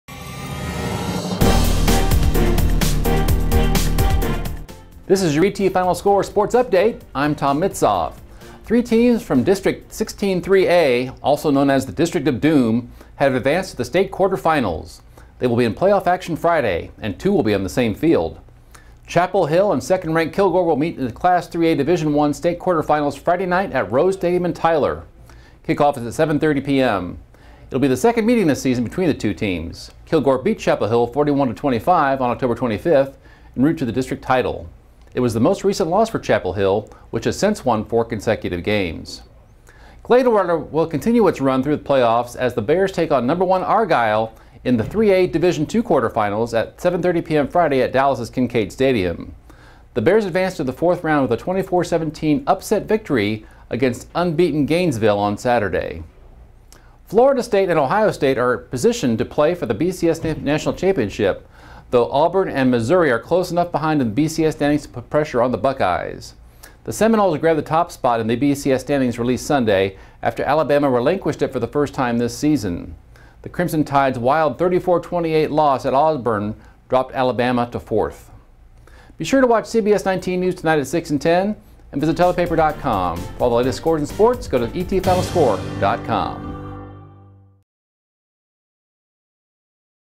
'District of Doom' has 3 teams in action Friday night
ETFinalScore.com Sports Update video for December 2, 2013